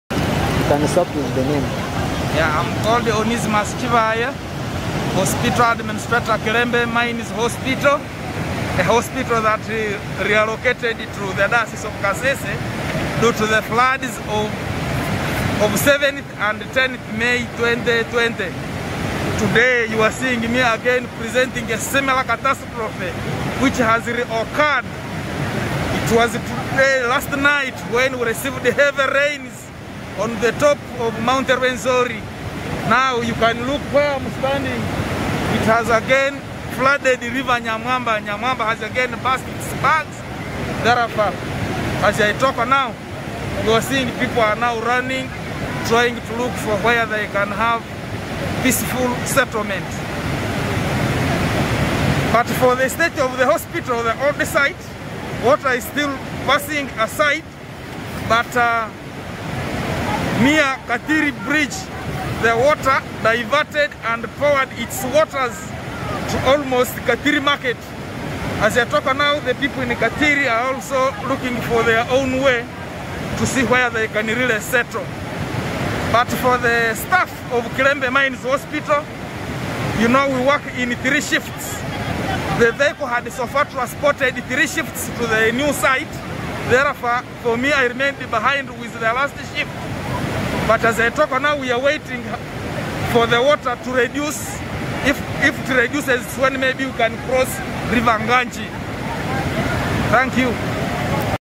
AMATABA GASAZEEKO ENGUUDO NOLUTINDO E KASESE
FLOODS IN KASESE HAVE PARALYZED TRANSPORT IN KASESE. EBYENTAMBUL;A E KASESE BISANNYALADDE.